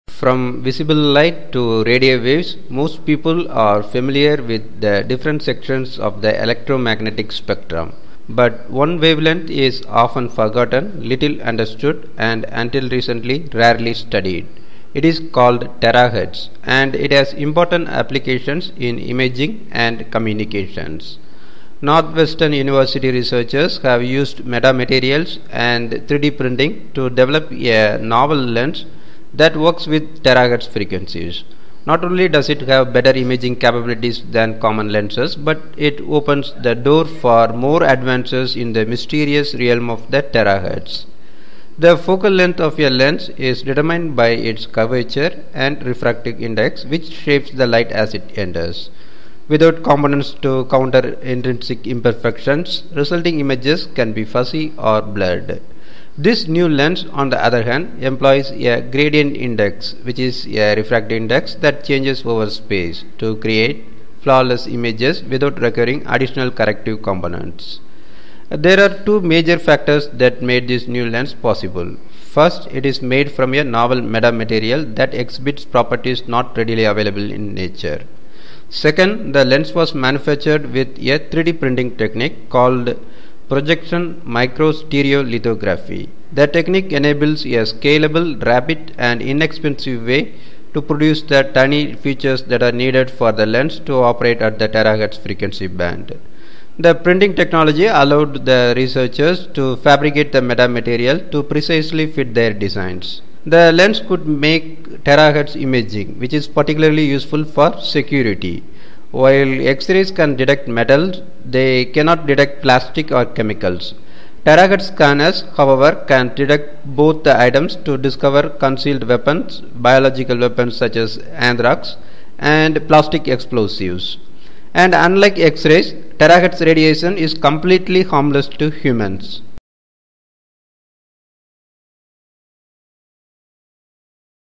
Light-powered 3-D printer creates terahertz lens
From visible light to radio waves, most people are familiar with the different sections of the electromagnetic spectrum. But one wavelength is often forgotten, little understood, and, until recently, rarely studied. It's called terahertz, and it has important applications in imaging and communications.

Northwestern University researchers have used metamaterials and 3Dprinting to develop a novel lens that works with terahertz frequencies. Not only does it have better imaging capabilities than common lenses, but it opens the door for more advances in the mysterious realm of the terahertz.

The focal length of a lens is determined by its curvature and refractive index, which shapes the light as it enters. Without components to counter intrinsic imperfections, resulting images can be fuzzy or blurred.  This new  lens, on the other hand, employs a gradient index, which is a refractive index that changes over space to create flawless images without requiring additional corrective components.

There are two major factors that made this new lens possible. First, it is made from a novel metamaterial that exhibits properties not readily available in nature.

Second, the lens was manufactured with a 3-D printing technique called projection micro-stereo-lithography. The technique enables a scalable, rapid, and inexpensive way to produce the tiny features that are needed for the lens to operate at the terahertz frequency band. The printing technology allowed the researchers to fabricate the metamaterial to precisely fit their designs.

The lens could make terahertz imaging, which is particularly useful for security.
While X-rays can detect metal, they cannot detect plastic or chemicals. Terahertz scanners, however, can detect both of items to discover concealed weapons, biological weapons such as anthrax, and plastic explosives. And unlike X-rays, terahertz radiation is completely harmless to humans.